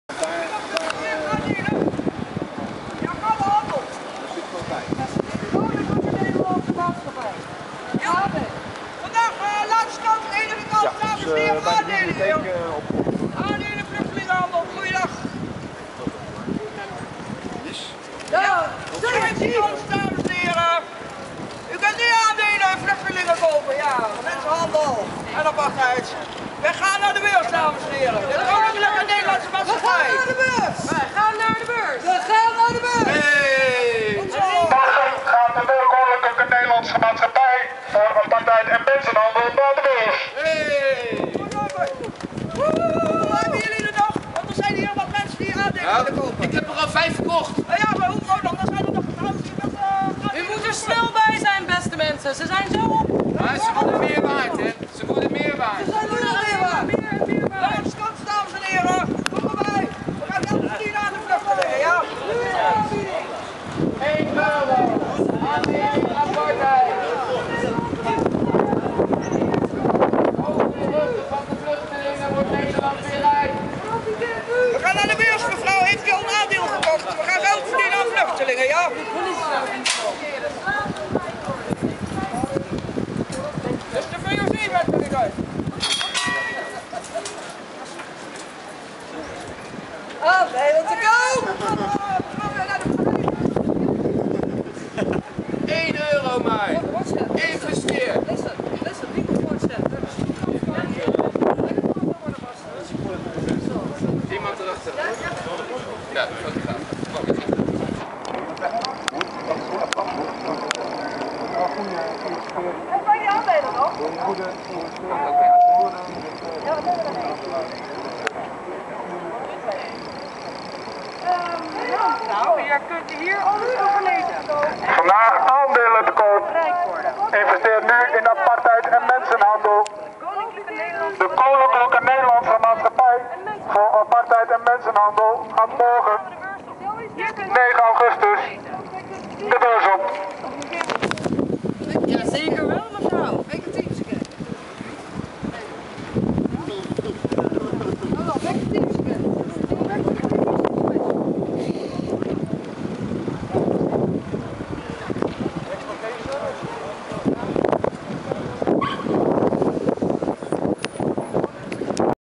No border 2013 Rotterdam
Een hoofdrol voor Elise Leijten.
Aandelenverkoop Apartheid en Mensenhandel in Rotterdam in 2013